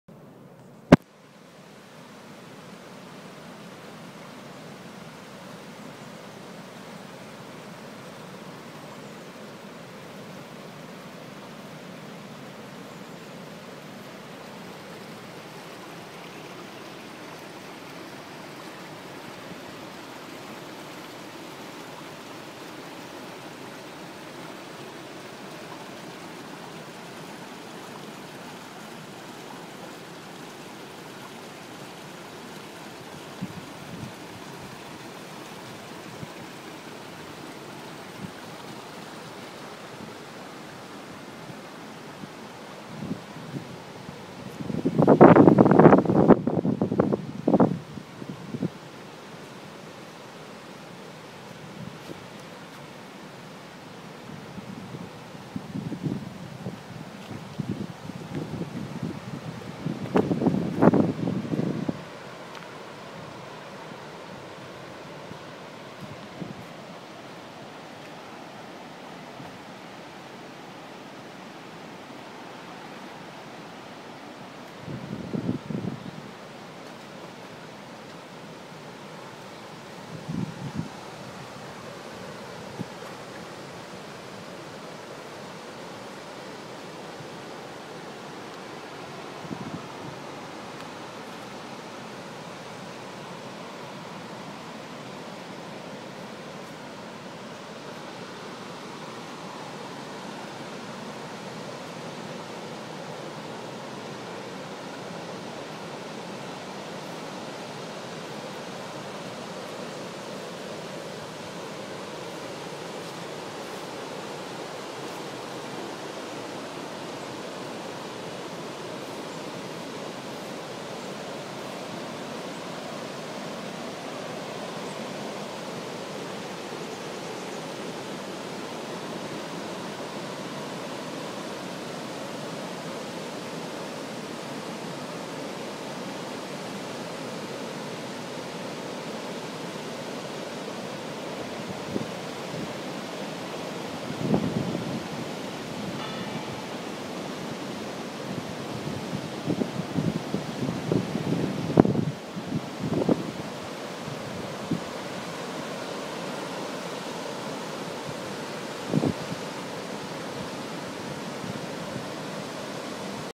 Moret sur Loing, ambiance médiévale qui en fait sa renommée.
Moret sur Loing, ambiance médiévale qui en fait sa renommée. Partez à la rencontre du patrimoine de la ville de Moret sur Loing, et plongez dans l’ambiance médiévale qui en fait sa renommée.